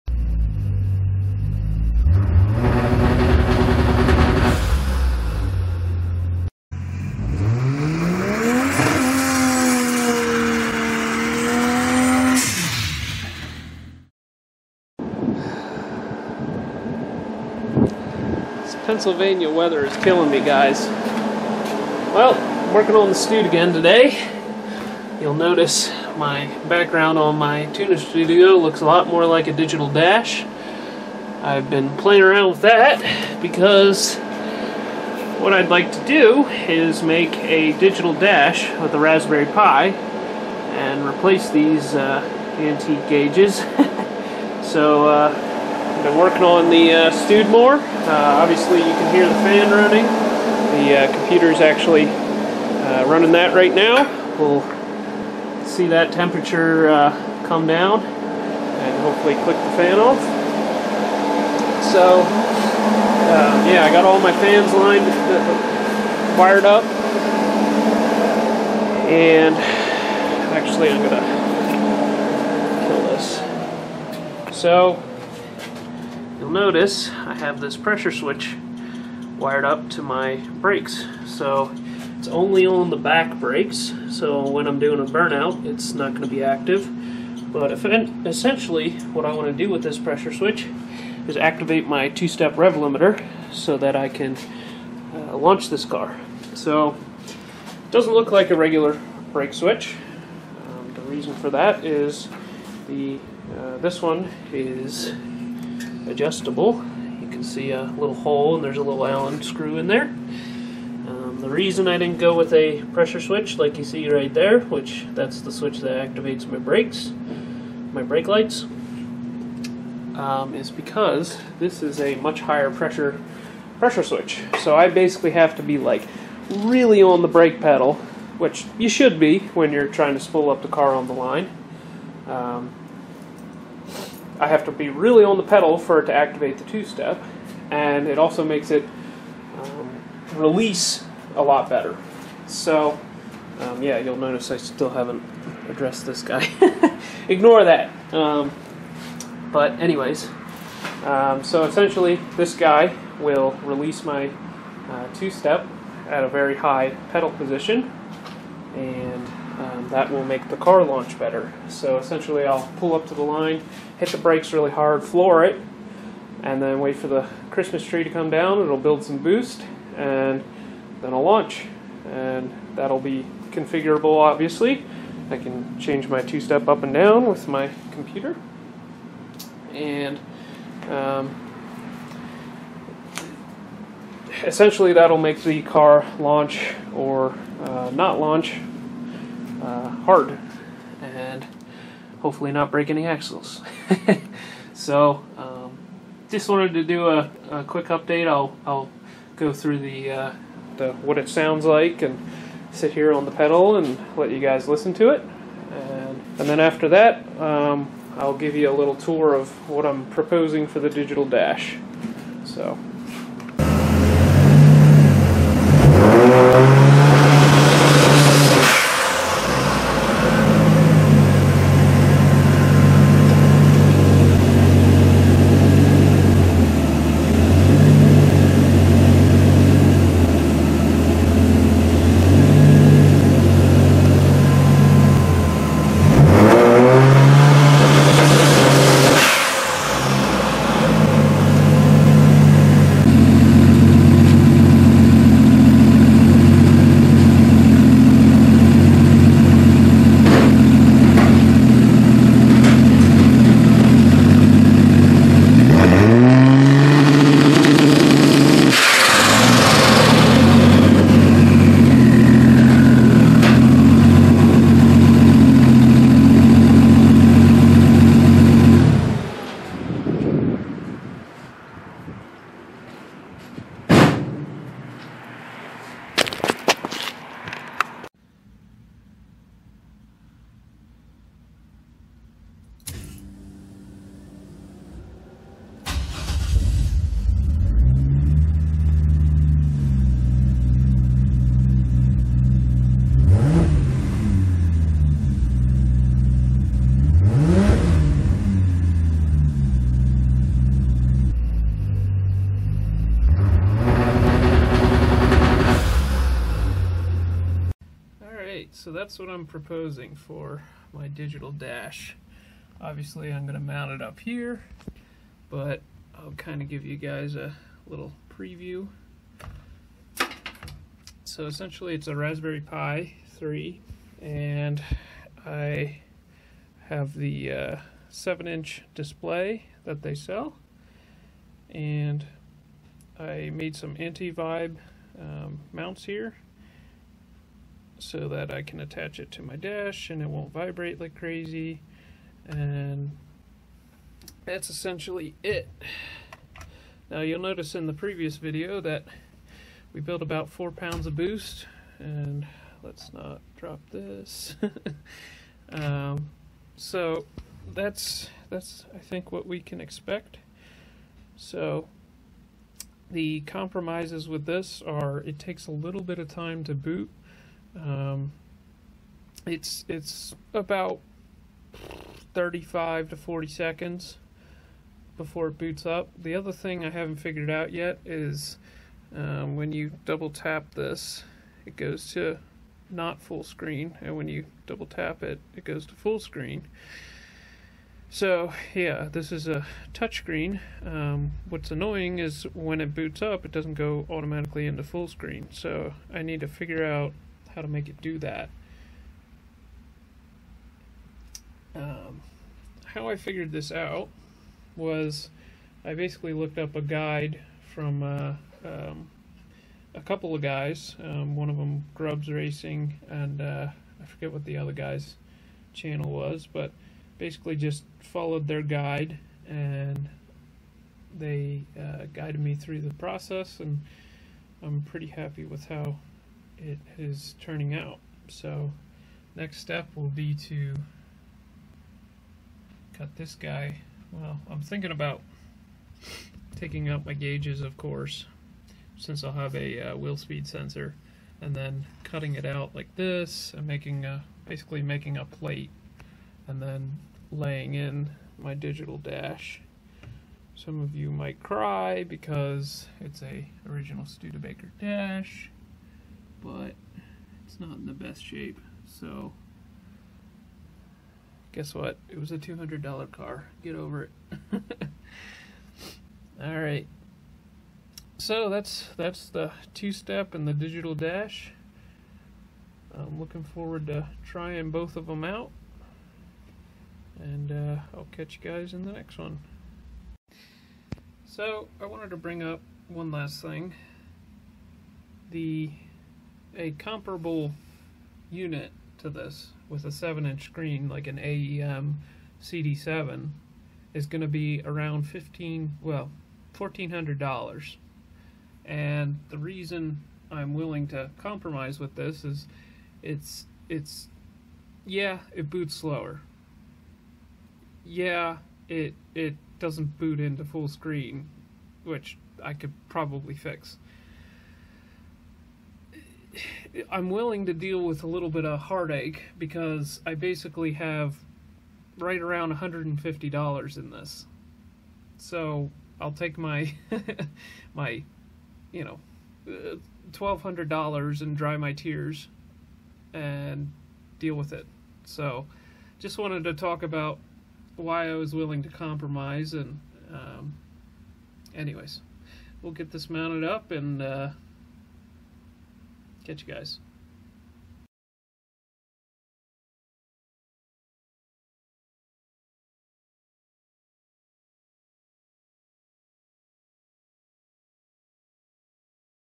Raspberry Pi Digital Dash + Hooking Up the 2 step
In today's video, we make a Raspberry Pi Digital Dash, and hookup the 2 step rev limiter using a brake pressure switch.